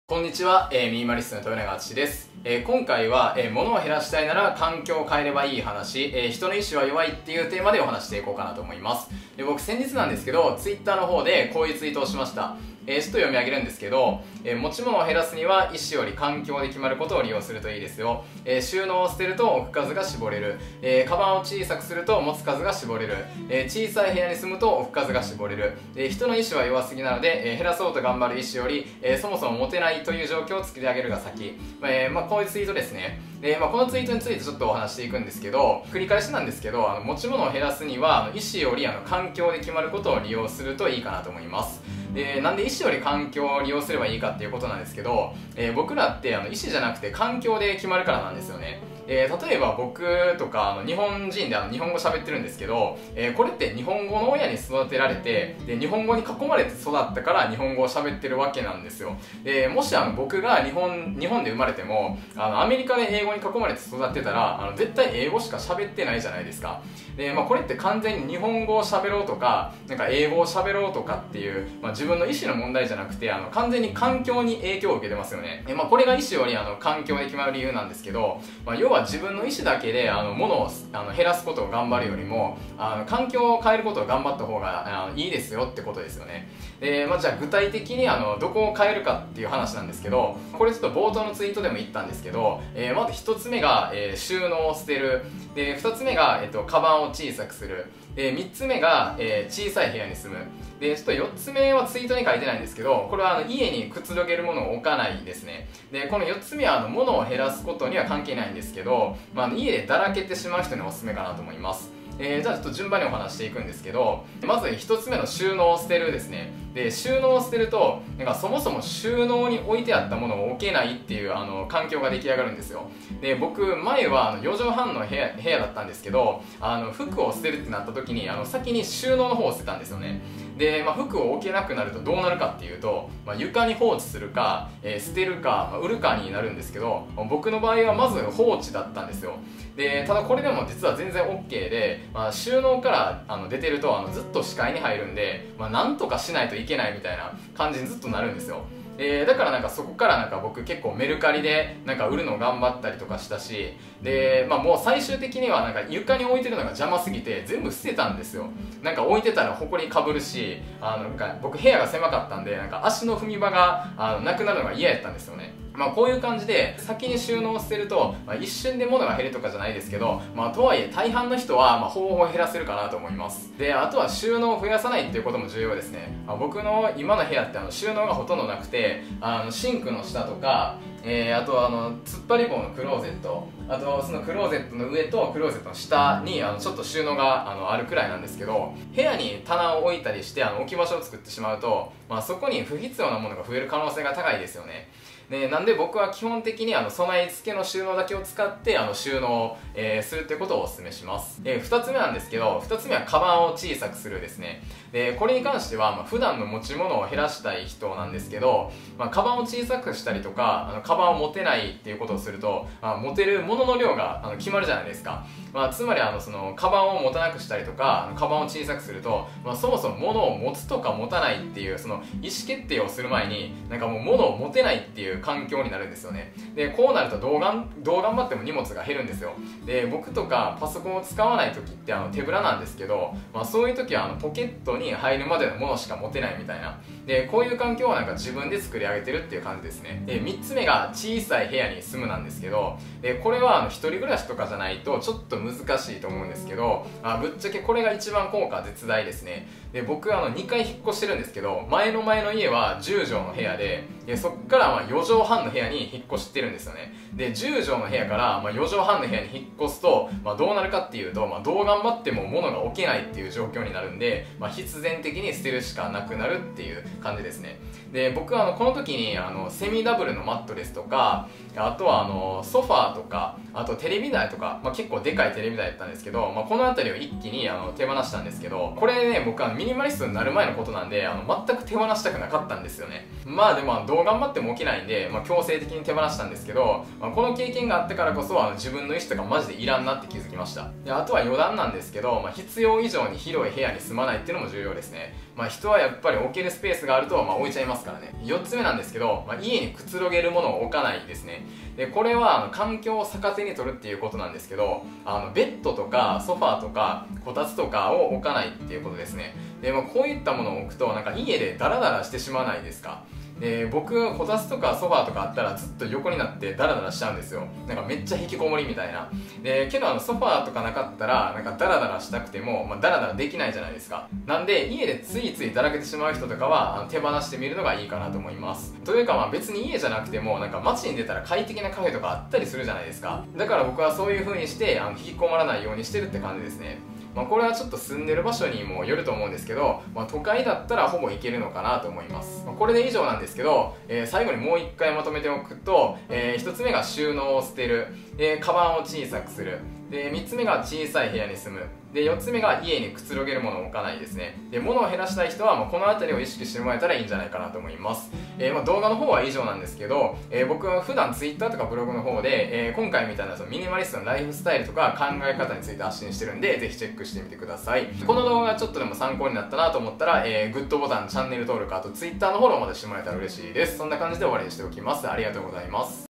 【ミニマリスト】モノを減らしたいなら、環境を変えればいい話【人の意思は弱い】
持ち物を減らすには、「意思」より「環境」で決まることを利用するといいですよ😌

✅収納を捨てる→置く数が絞れる
✅鞄を小さくする→持つ数が絞れる
✅小さい部屋に住む→置く数が絞れる

人の意思は弱すぎなので、「減らそうと頑張る意思」より、「そもそも持てない」という状態を作り上げるが先。

大阪のコンパクトな部屋に住んでいるミニマリストです。
Twitterとブログでは、ミニマリストのライフスタイル、思考について発信しているので、ぜひチェックしてみてください😌